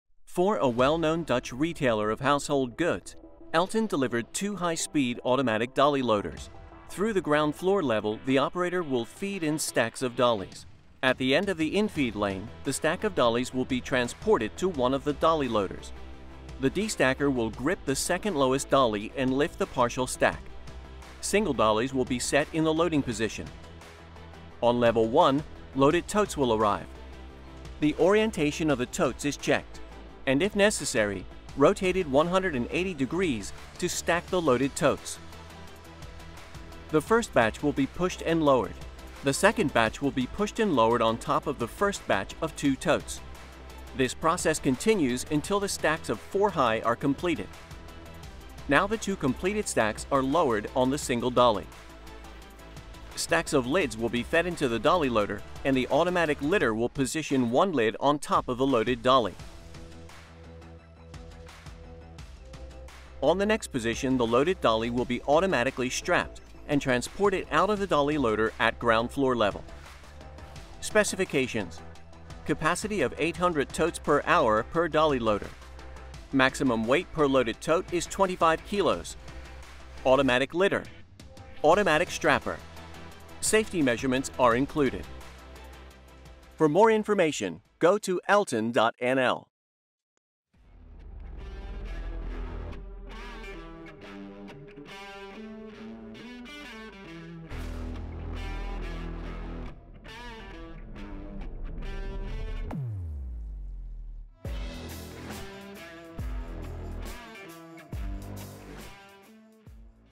Automatic Dolly Loader – Household Goods | Handling machines | Elten Logistic Systems B.V.
For a well-known Dutch retailer of household goods, Elten delivered two high
 speed Automatic Dolly Loaders.
Through the ground floor level the Operator will feed in stacks of Dollies.

 At the end of the Infeed Lane the stack of Dollies will be transported to one of
 the Dolly Loaders.

 The Destacker will grip the 2nd lowest Dolly and lift the partial stack. Single
 Dollies will be set in the loading position.
 On level 1, loaded totes will arrive. The orientation of the Tote is checked and, if
 necessary, rotated 180 degrees to stack the loaded Totes.
 The 1st batch will be pushed and lowered. The 2nd batch will be pushed and
 lowered on top of the 1st batch of two Totes. This process continues until the
 Stacks of 4 high are completed.

 Now the two completed stacks are lowered on the single Dolly.
 Stacks of Lids will be fed into the Dolly Loader and the Automatic Lidder will
 position 1 Lid on top of the loaded Dolly.
 On the next position the loaded Dolly will be automatically strapped and
 transported out of the Dolly Loader at ground floor level.

 Specifications:
Capacity of 800 Totes per hour per Dolly Loader.
Maximum weight per loaded Tote is 25kg.
Automatic Lidder.
Automatic Strapper.
Safety Measures are included.